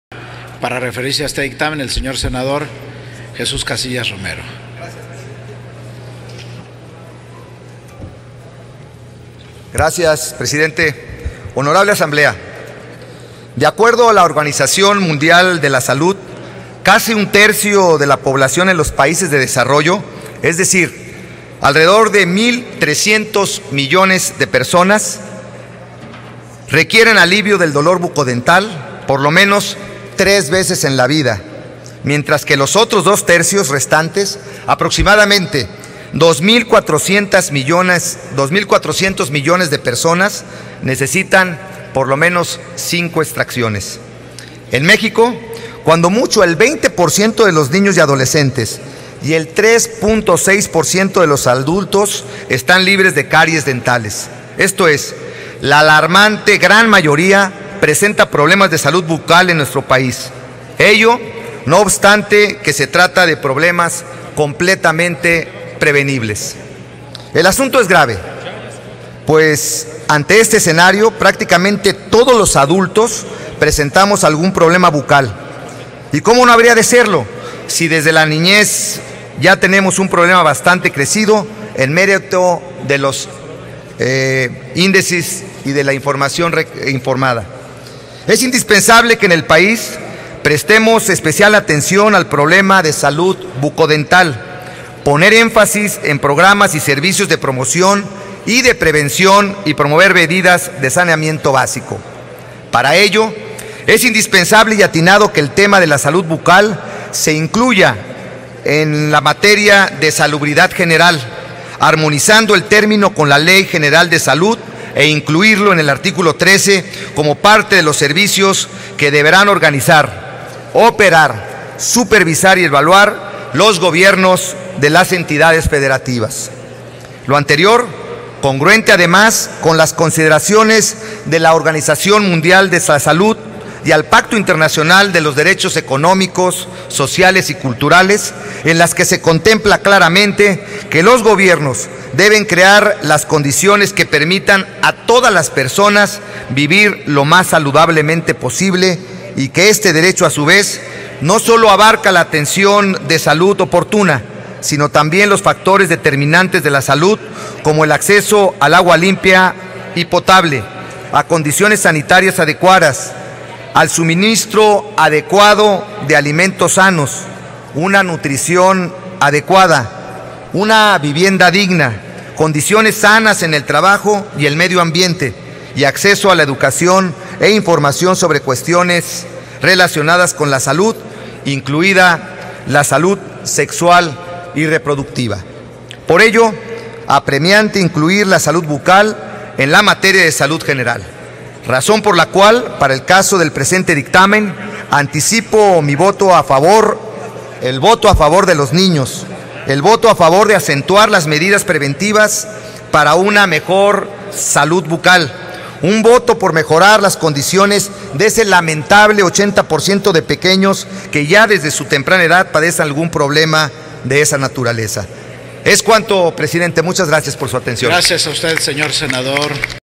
Es indispensable que en el país prestemos atención a la salud bucodental: Sen. Jesús Casillas
Sen. Jesús Casillas habla a favor del dictamen en materia de salud bucodental. Es indispensable que en el país prestemos atención a la salud bucodental, destacó el senador.